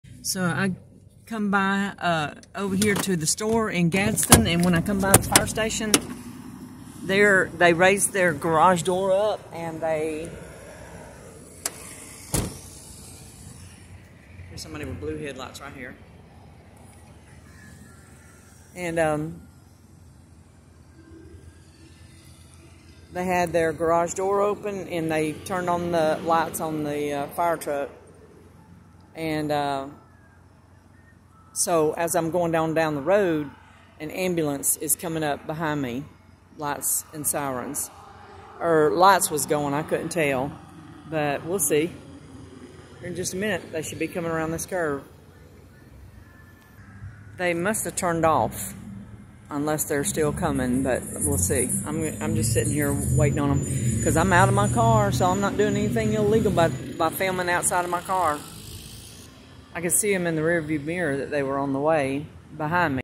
🚒🚑🎪🎭9.30.23 - 21:08 - Gadsden AL EMS Follow Me Through Gadsden & Inside Walmart.
the reason they didn't drive by after following me on Meighan Blvd, is bc they turned off into Walmart Parking Lot and followed me into the store. so they had lights going to chase me, turned them off, came into Walmart Parking Lot and followed me in the store by the Pharmacy.